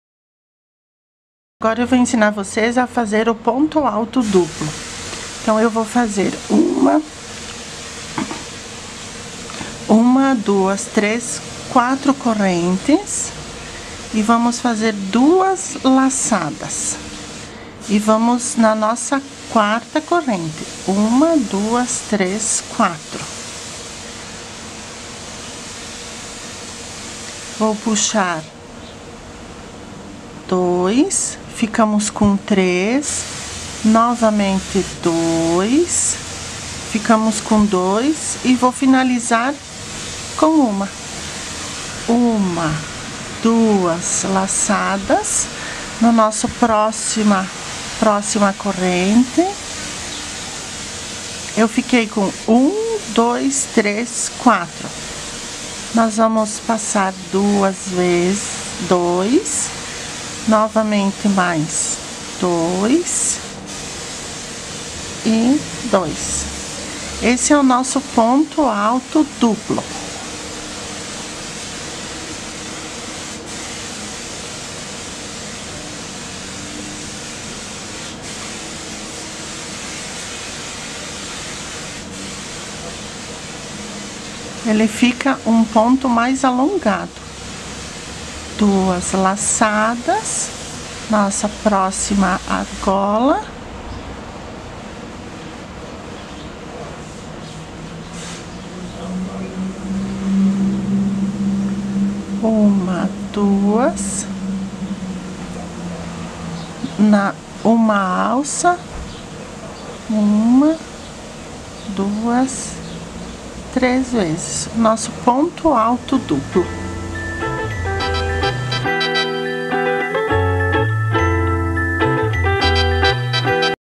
Crochê para Iniciantes - Ponto Alto Duplo
Nesta nossa nova série de vídeos Crochê para Iniciantes, você vai aprender os pontos básicos de crochê para iniciar seus trabalhos e conseguir desenvolver as peças que apresentamos nos vídeos.

Dúvidas? Escreva pra gente!